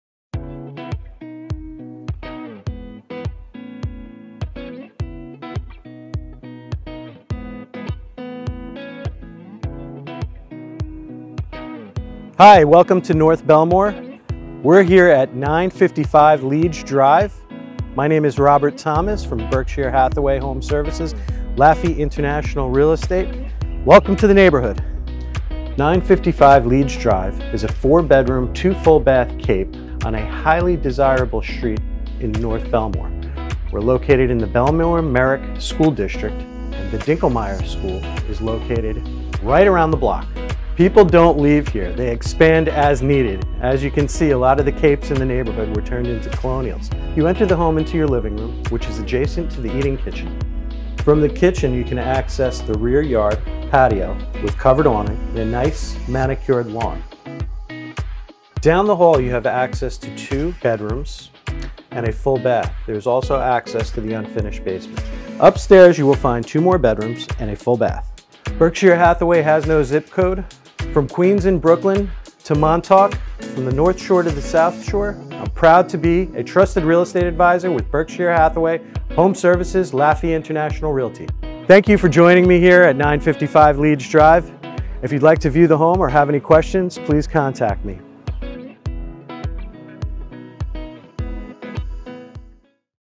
Welcome to 955 Leeds Dr, Bellmore, NY Virtual Tour | Long Island Homes for Sale
Robert V. Thomas shows us the possibilities of 955 Leeds Dr in Bellmore, NY. This quaint 4 bedroom cape in the desirable section of North Bellmore is located around the corner from the Dinkelmeyer Elementary School.

ROBERT THOMAS
Licensed Real Estate Salesperson